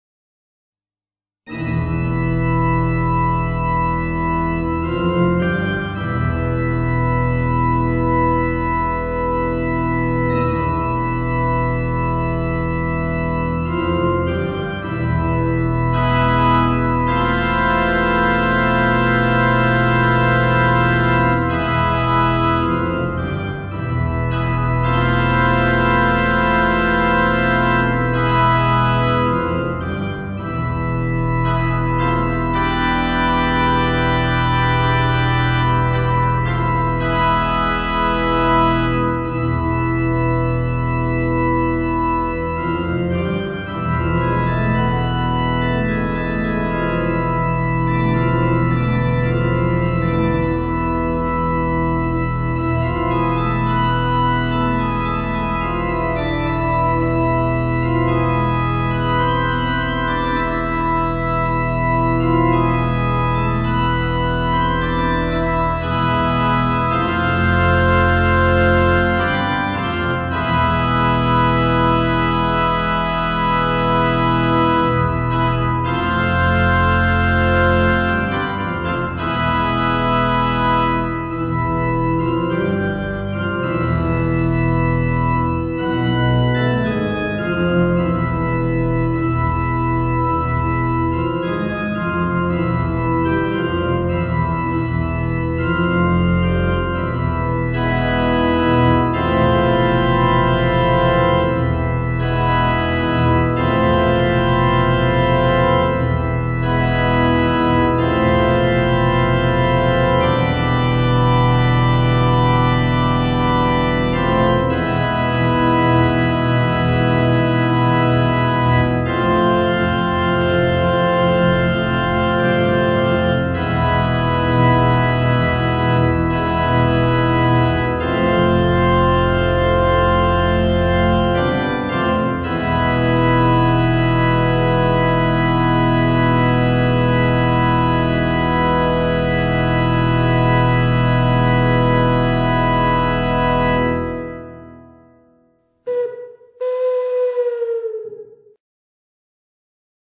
The Owl plays the organ
Kosherbear has owls and pays them for playing the organ and hunting rats. ;-) Here Owl Dr. Nunu is improvising. ;-)